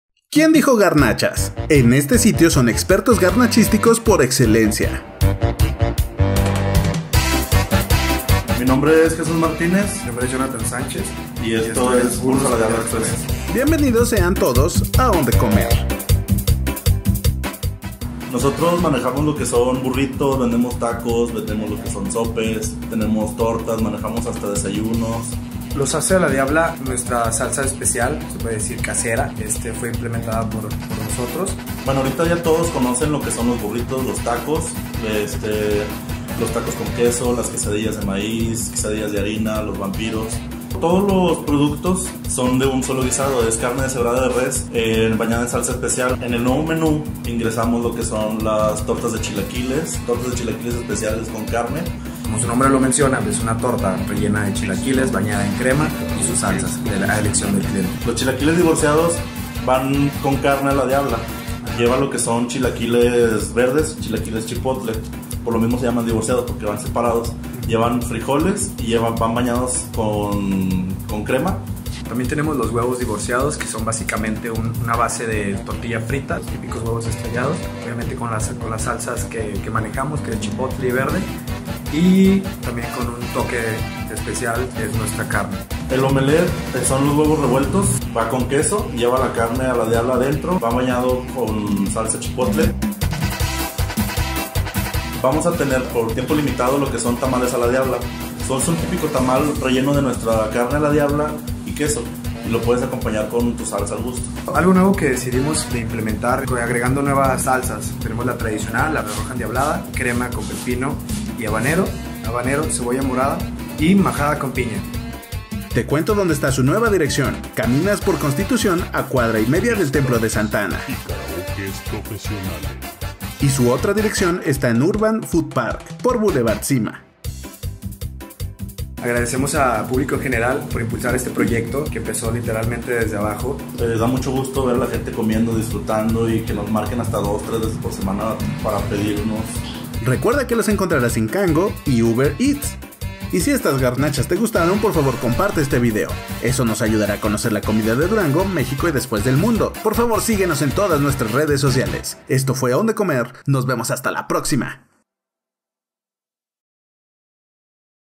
Son deliciosos y siempre a la diabla
Estamos seguros que nuestros Tragonautas serán totalmente felices en este lugar porque todo tiene el toque especial que sólo Burros a la diabla express le sabe dar, ahora con nuevos platillos.

Están en: Calle Constitución #450 entre gómez palacio y pereyra.
Y en Urban Food Park , Av. La Cima #315. (sólo platillos seleccionados)

CONOCE MÁS:
-TIPO DE ESTABLECIMIENTO: Restaurante
-ESTACIONAMIENTO: Calle
-PAGOS: No
-BEBIDAS ALCOHÓLICAS: En Constitución no, Urban Food park si.
-SERVICIO A DOMICILIO: Uber Eats- Cango
-OPCIÓN VEGETARIANA: Si